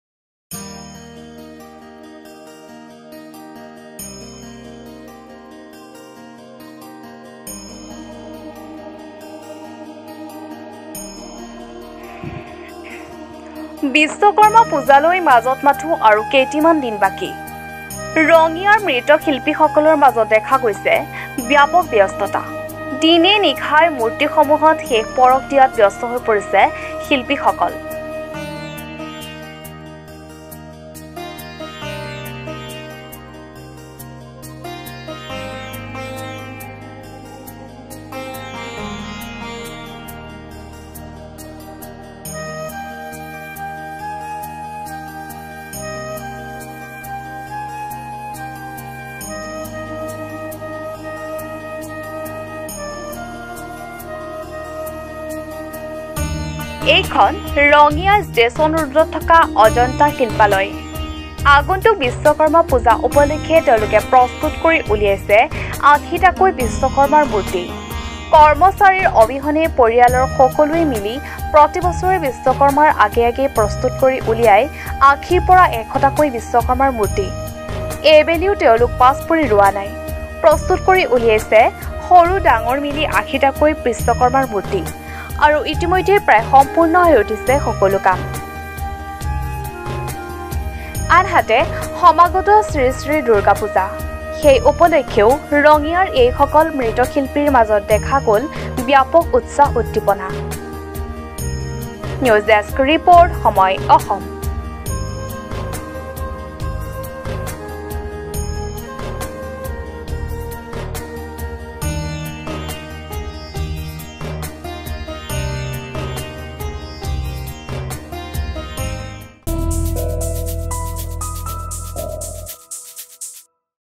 📌বিশ্ব কৰ্মা পূজালৈ মাজত মাথো আৰু কেইটা মান দিন বাকী
📌ৰঙিয়াৰ মৃৎশিল্পী সকলৰ মাজত দেখা গৈছে ব্যাপক ব্যস্ততা।